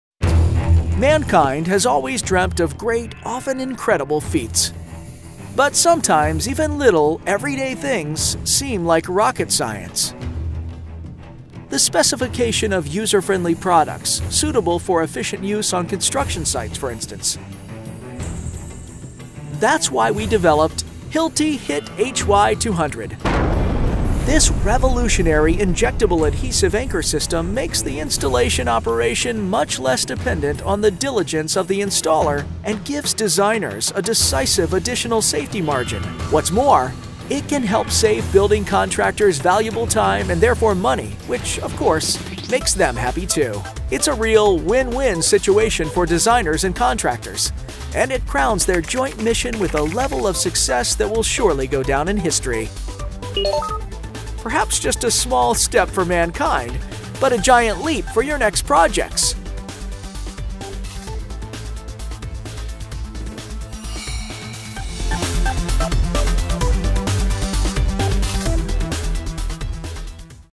INTRODUCING the Hilti HIT-HY 200 Adhesive Anchor - one giant leap
Want to learn more? Canada

Now you can install anchor rod and post-installed rebar connections with more confidence. Inadequately cleaning holes during installation can reduce the performance of conventional chemical anchor systems significantly. Hilti Safe Set™ Technology eliminates this factor almost entirely -- in both cracked or uncracked concrete and with anchor rods or post-installed rebar.